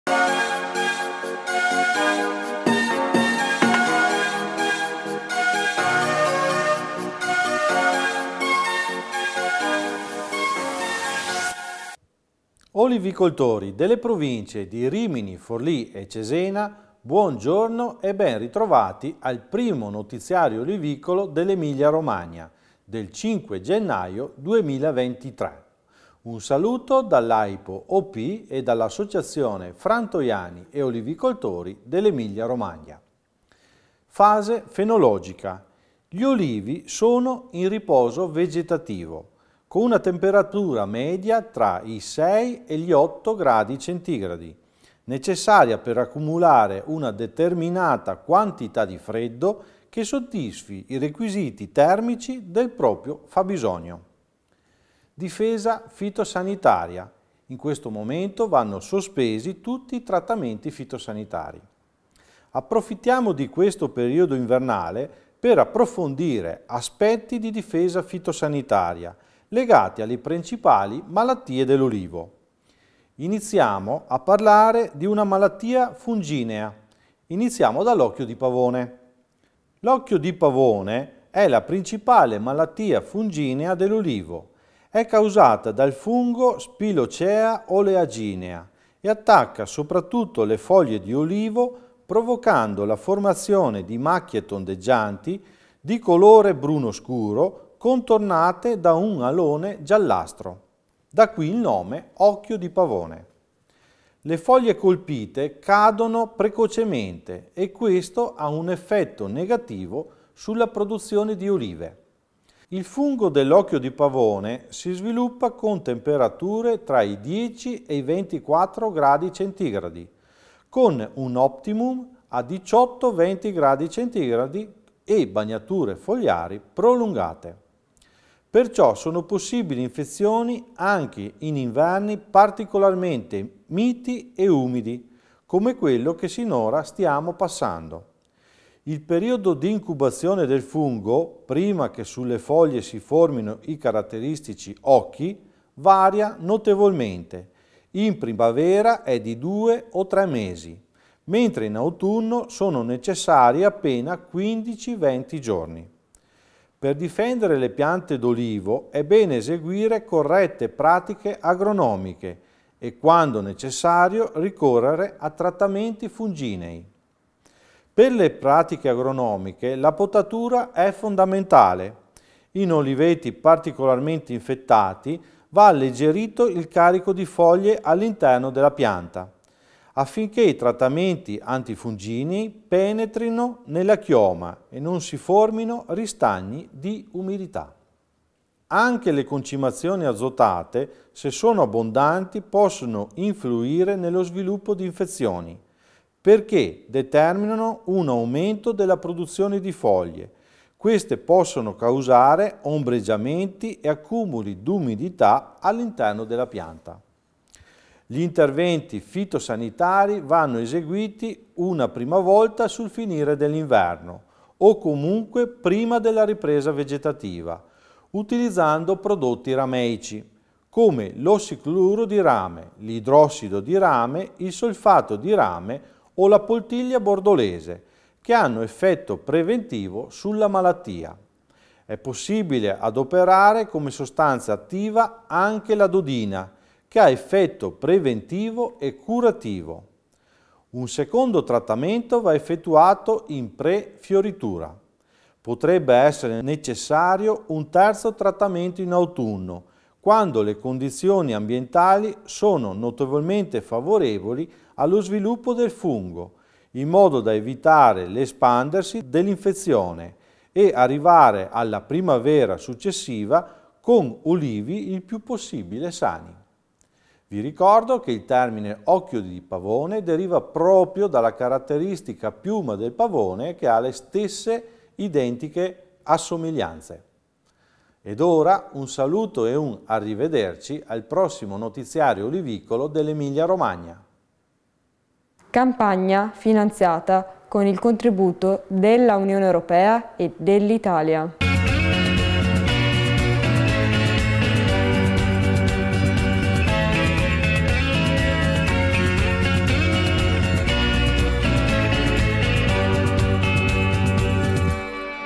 Notiziario olivicolo Emilia Romagna 5 gennaio 2023
Notiziario olivicolo dell’Emilia Romagna n 1 del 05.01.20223
per essere costantemente aggiornati,

Fase fenologica: gli olivi sono in riposo vegetativo, con una temperatura media tra 6 e 8°C, necessaria per accumulare una determinata “quantità di freddo”, che soddisfi i requisiti termici del proprio fabbisogno. Difesa fitosanitaria: sospensione trattamenti.
Approfittiamo di questo periodo invernale per approfondire aspetti di difesa fitosanitaria legati alle principali malattie dell’olivo. Iniziamo a “parlare” di malattie fungine iniziamo dall’Occhio di pavone. L'Occhio di pavone è la principale malattia fungina dell’olivo, è causata dal fungo Spilocaea oleaginea e attacca soprattutto le foglie di olivo provocando la formazione di macchie tondeggianti, di colore bruno scuro, contornate da un alone giallastro. Da qui il nome di occhio di pavone.
Le foglie colpite cadono precocemente e, questo, ha un effetto negativo sulla produzione di olive. Il fungo dell'Occhio di pavone si sviluppa con temperatura tra i 10° e 24° C, con un optimum a 18-20°C e on bagnature fogliari prolungate.
Perciò sono possibili infezioni anche in inverni particolarmente miti e umidi, come quello che sinora stiamo passando. Il periodo d’incubazione del fungo prima che sulle foglie si formino i caratteristici “gli occhi”, varia notevolmente, in primavera è di 2 o 3 mesi, mentre in autunno sono necessari appena 15/20 giorni. Per difendere le piante d’olivo è bene eseguire corrette pratiche agronomiche e, quando necessario, ricorrere a trattamenti fungicidi.
Per le pratiche agronomiche la potatura è fondamentale, in oliveti particolarmente infettati va alleggerito il carico di foglie all'interno della pianta, affinché i trattamenti antifungini penetrino nella chioma e non si formino ristagni di umidità.
Anche le concimazioni azotate, se sono abbondanti, possono influire nello sviluppo d’infezioni, perché, determinando un aumento della produzione di foglie, queste possono causare ombreggiamenti e accumuli d’umidità all'interno della pianta.
Gli interventi fitosanitari vanno eseguiti una prima volta sul finire dell'inverno o, comunque, prima della ripresa vegetativa, utilizzando prodotti rameici, come l’ossicloruro di rame, l’idrossido di rame, il solfato di rame o la poltiglia bordolese, che hanno effetto preventivo sulla malattia.
È possibile adoperare come sostanza attiva anche la Dodina, che ha effetto preventivo e curativo.
Un secondo trattamento va effettuato in pre-fioritura.
Potrebbe essere necessario un terzo trattamento in autunno, quando le condizioni ambientali sono nuovamente favorevoli allo sviluppo del fungo, in modo da evitare l'espandersi dell'infezione e arrivare alla primavera successiva con olivi il più possibile sani.